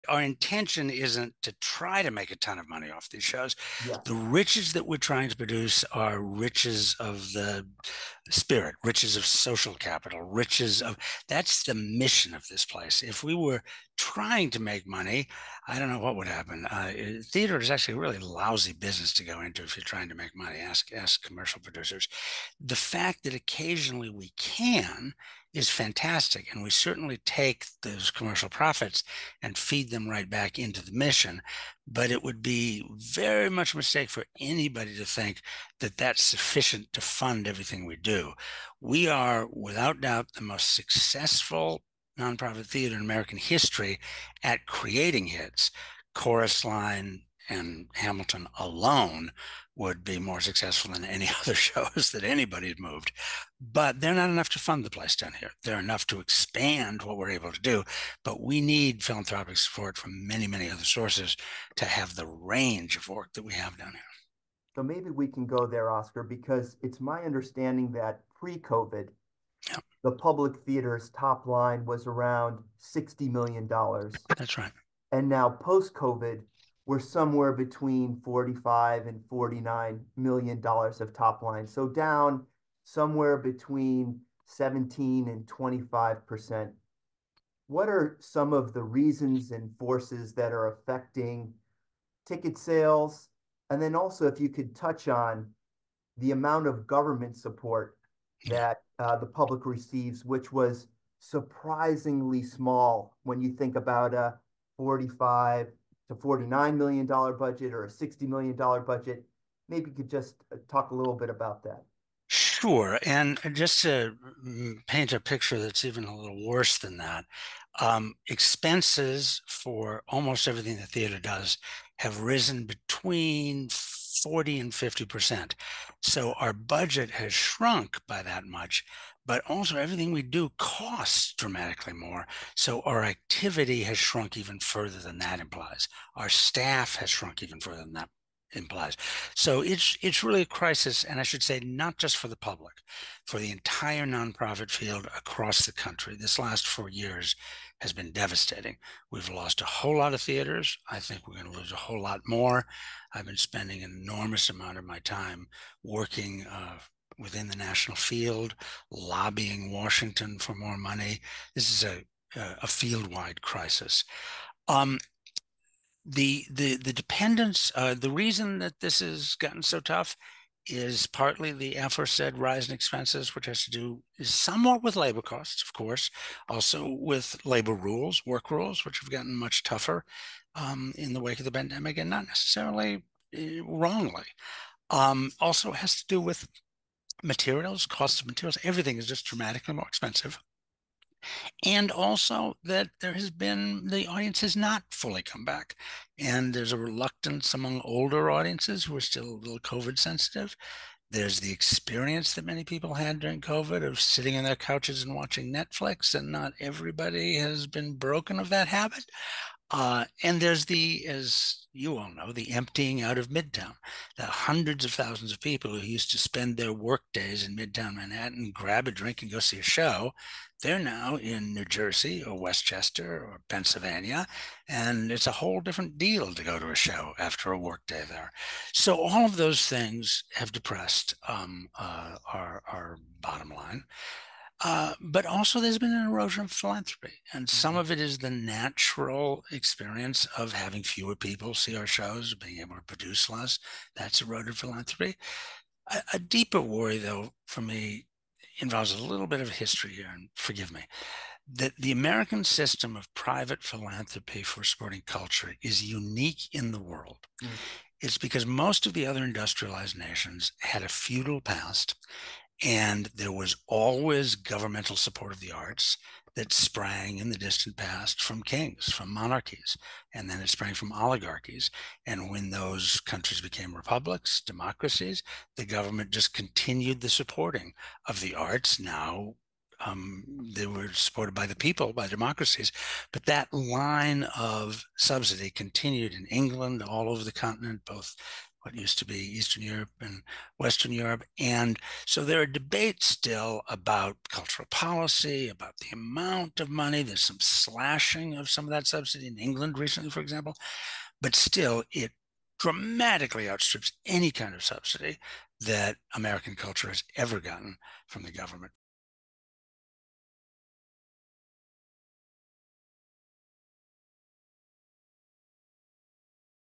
Oskar Eustis discusses philanthropic culture in America and the funding of The Public Theater
Oskar Eustis, Artistic Director of The Public Theater, joined the Economic Club of New York for a conversation on the transformative power of the arts in fostering economic development and serving as a catalyst for social change.

763rd Meeting of the Economic Club of New York
Moderated by Andrew Klaber, CEO of Bedford Ridge Capital
Presided by Barbara Van Allen, President & CEO of the Economic Club of New York